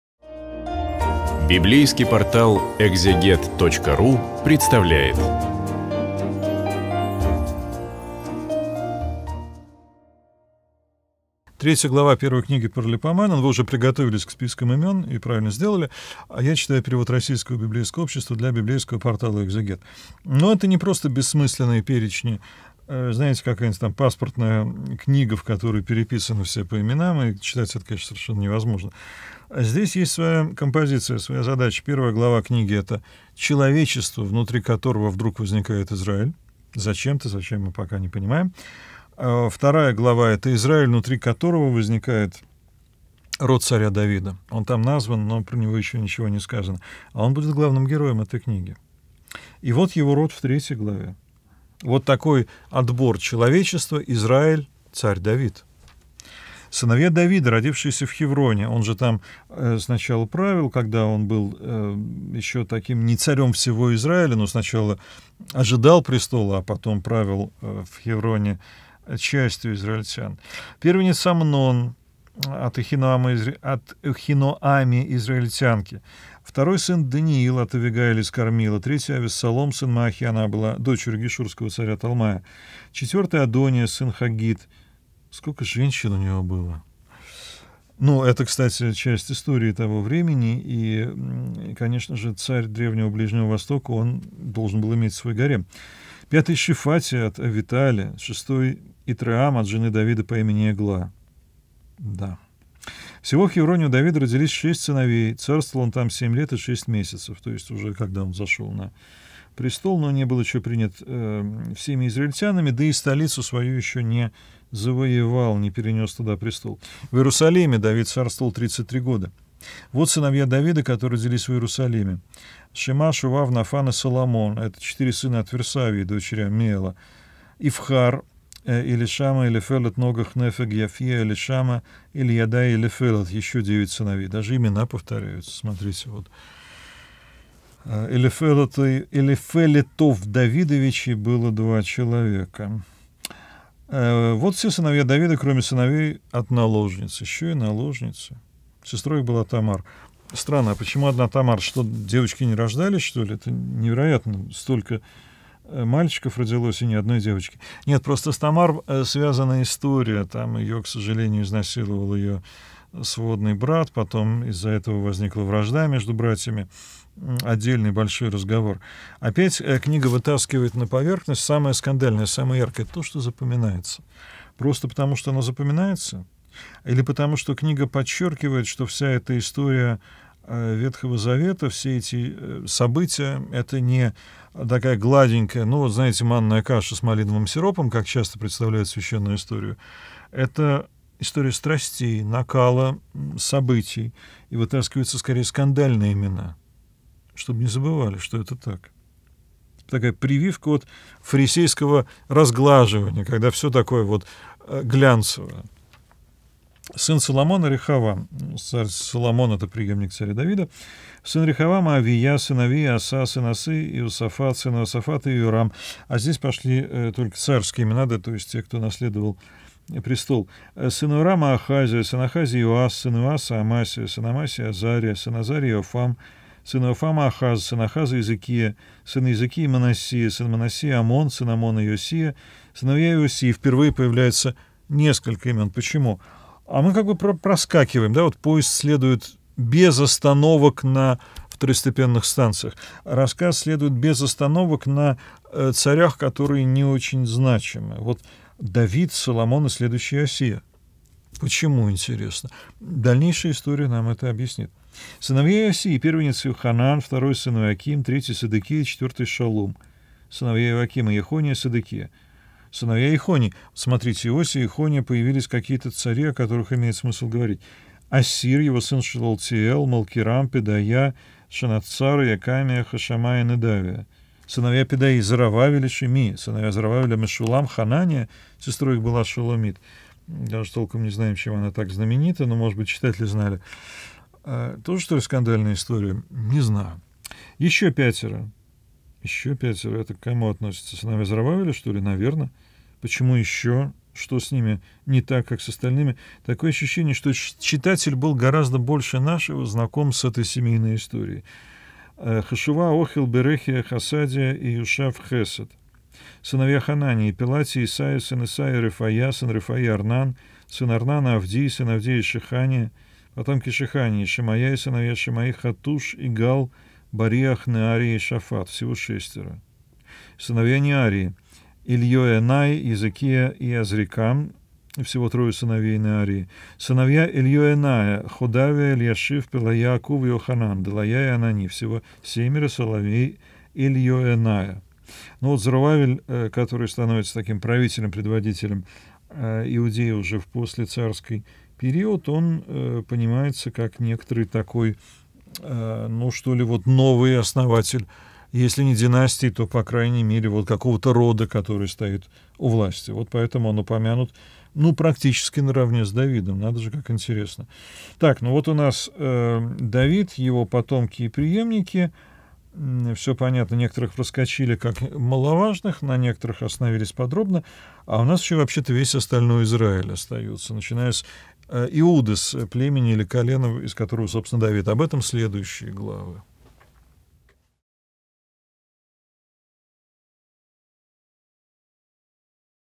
1-я Книга Паралипоменон. Глава 3. Андрей Десницкий. Ветхий Завет
В третьей главе мы читаем про род царя Давида. Идёт перечисление жён и сыновей, которых Давид родил. Также упоминается и его дочь Фамарь, которая была обесчещена своим единокровным братом Амноном. Опять мы видит неприглядную картину, в который выносится на поверхность все самое грязное и гнусное.